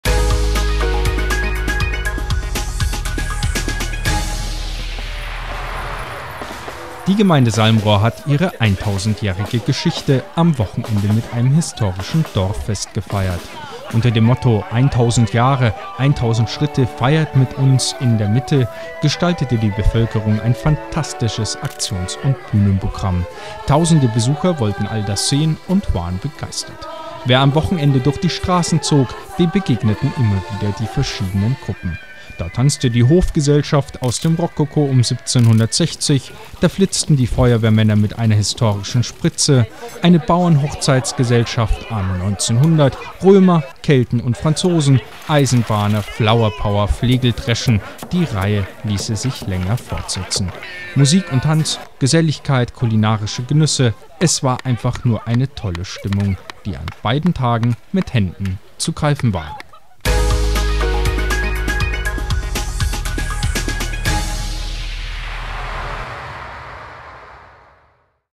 volksfreund tv: 1000 Jahre Salmrohr
Die Gemeinde Salmrohr hat ihre 1000-jährige Geschichte am Wochenende mit einem historischen Dorffest gefeiert.